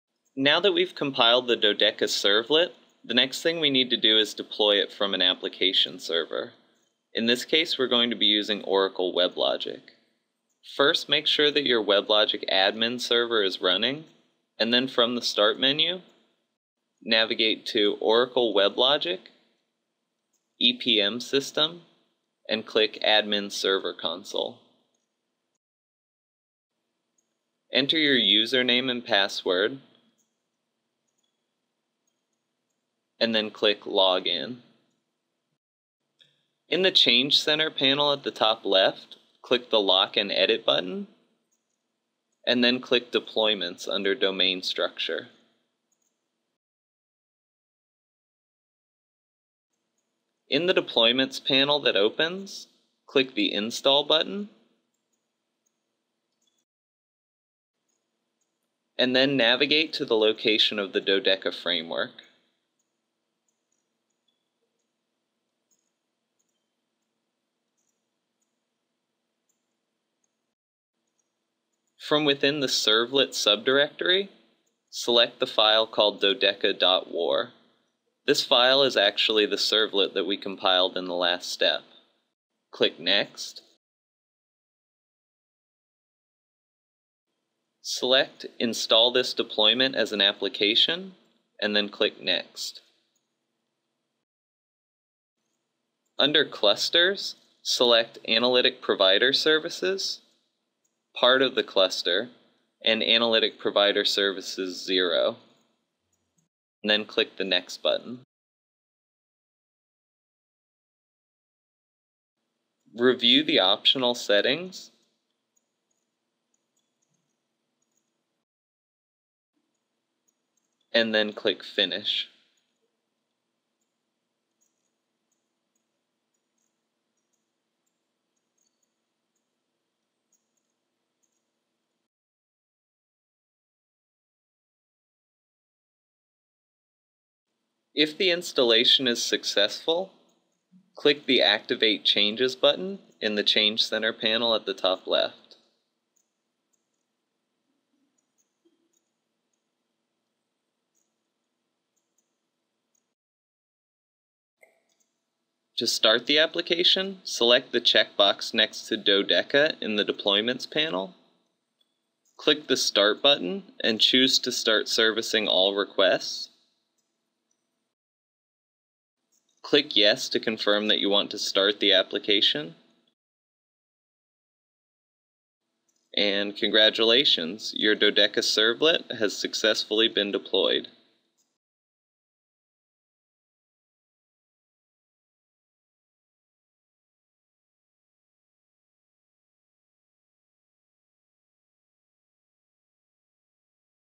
Deploying the Dodeca Servlet - Oracle WebLogic (Installing Dodeca, Part 5/5)
Dodeca is enterprise-class software that dramatically increases the speed, reliability, and accuracy of business analysis, planning, and reporting. This is part 5 of 5 in the "Installing Dodeca" video series, showing you how to deploy the Dodeca servlet to an Oracle WebLogic server. You can view all the videos in the series directly from our playlist,